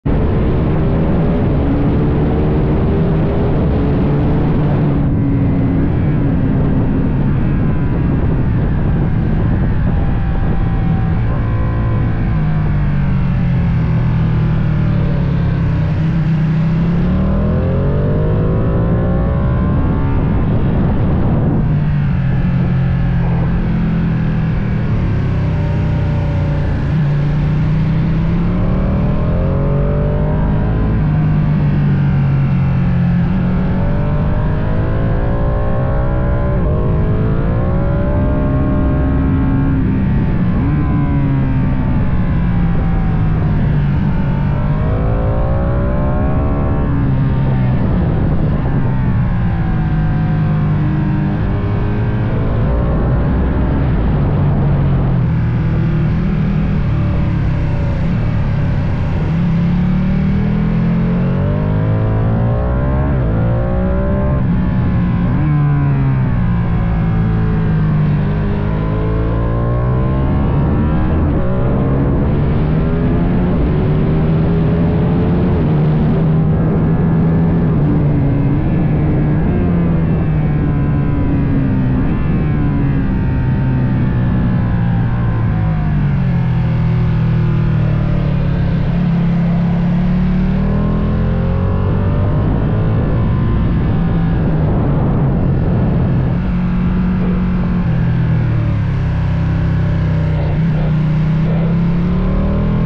Insta360 X4 DEWARP - Duati Panigale V2 on SUZUKA Twin RIRELLI fun Track day
6月8日に鈴鹿ツインサーキットで開催されたピレリ・ファン・トラックデーで走行したDUCATI Panigale V2にInsta360 X4を装着して撮影しました。
でInsta360 Studioで正面動画をDEWARPモード、画角90度で書き出してみました。